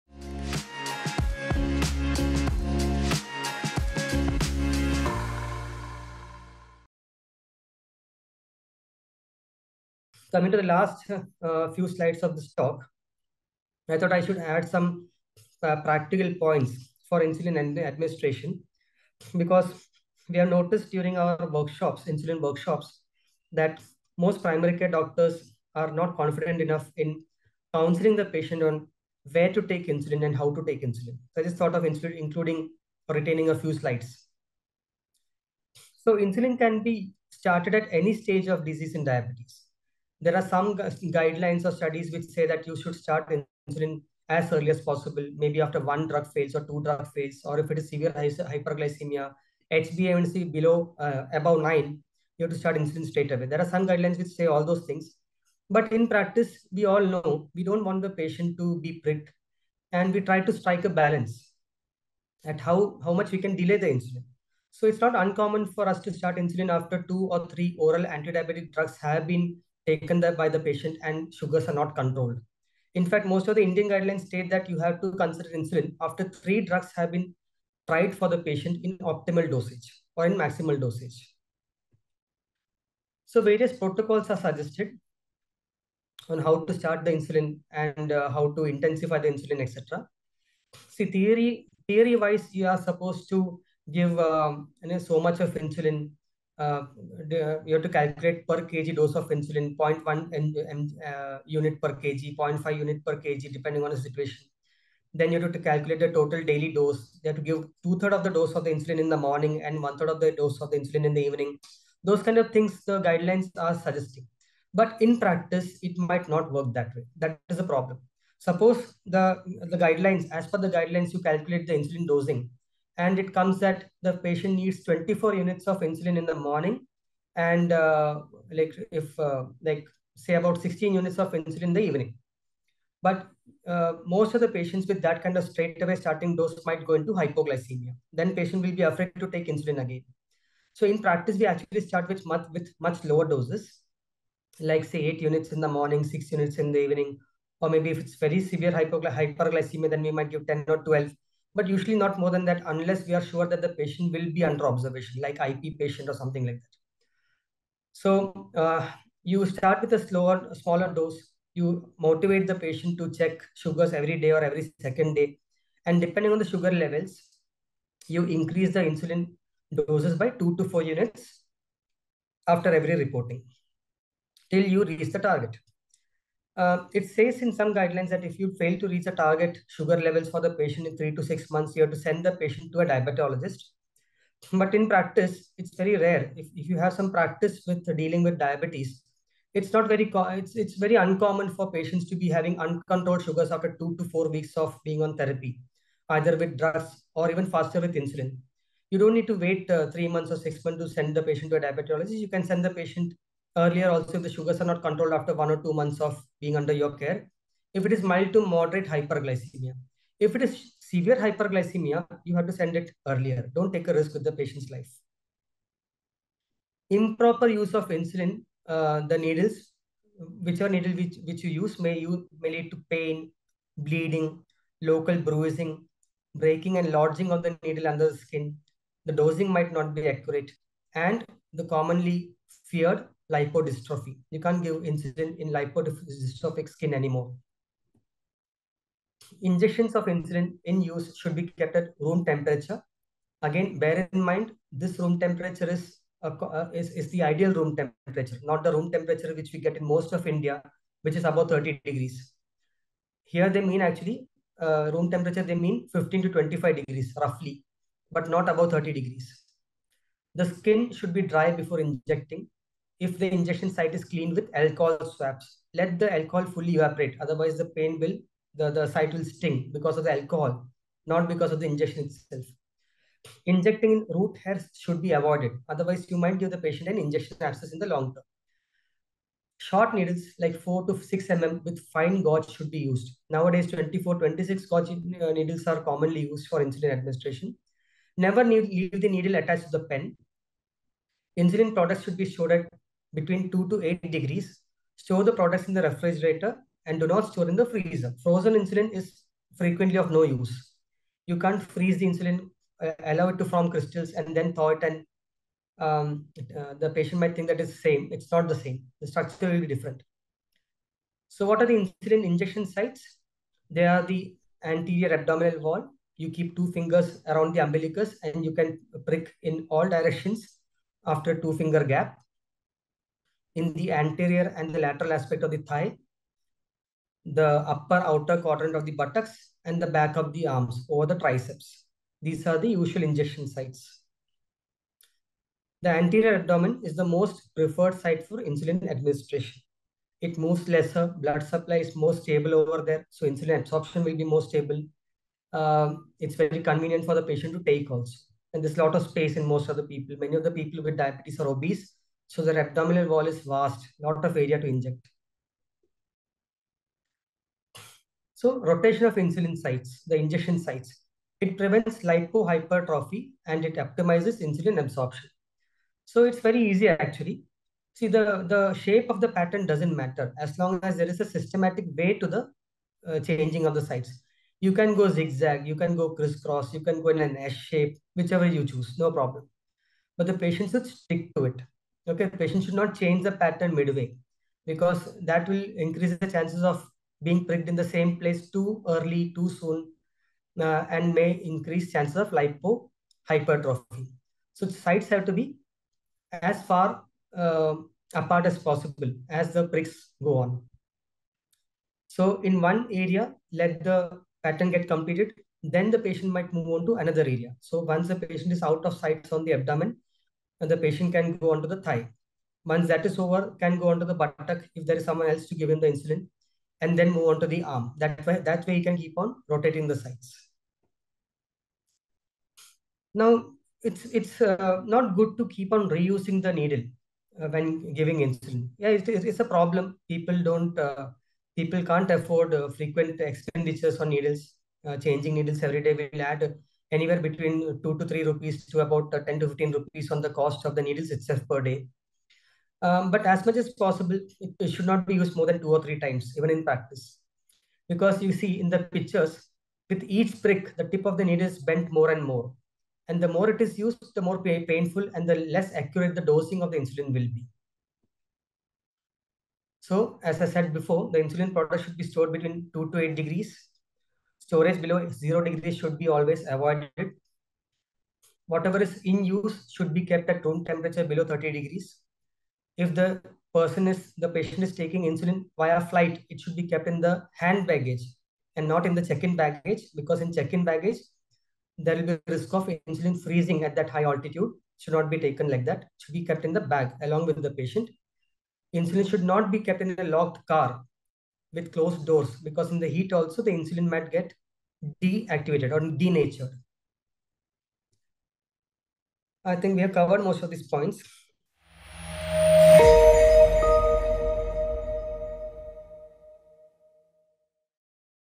Clinical and Practical Nuggets for Insulin Administration | Medical Case Discussion
When administering insulin, it's essential to ensure proper technique to optimize its effectiveness and minimize complications. Patients should be educated on the importance of rotating injection sites within the same anatomical region to prevent lipohypertrophy, a condition characterized by fat accumulation at injection sites that can affect insulin absorption.

Regular monitoring of blood glucose levels, along with clear communication between patients and healthcare providers, is essential to adjust insulin doses appropriately and achieve optimal glycemic control while minimizing the risk of hypoglycemia. Additionally, patients should be educated on the signs and symptoms of hypoglycemia and how to treat it promptly with glucose-containing foods or beverages.

Dr. Bijayraj. R, Sr. Consultant, Family Medicine and Diabetology, Starcare Hospital, Kerala, explains to us the clinical and practical nuggets for insulin administration.